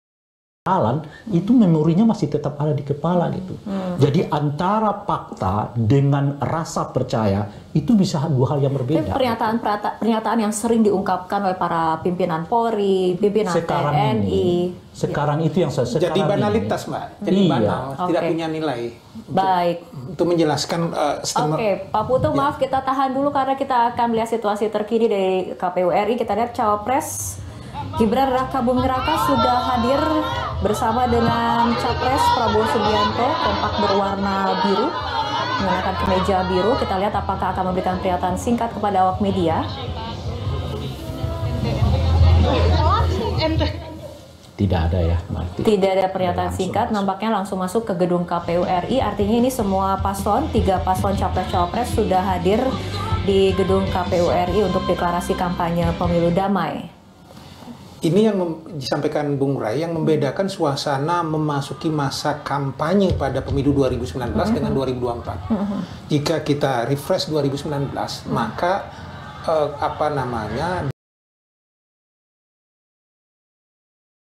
HADIRI DEKLARASI PEMILU DAMAI 2024, PRABOWO-GIBRAN TIBA DI GEDUNG KPU
Komisi Pemilihan Umum menggelar Deklarasi Pemilu Damai untuk Pemilihan Umum atau Pemilu 2024, Senin (27/11/2023). Paslon capres-cawapres nomor urut 2, Prabowo Subianto dan Gibran Rakabuming Raja tiba di Gedung KPU RI, Jakarta Pusat. Prabowo-Gibran datang ke Gedung KPU bersama-sama dan kompak mengenakan kemeja warna biru. Prabowo-Gibran tiba di Gedung KPU sekitar pukul 14.45 WIB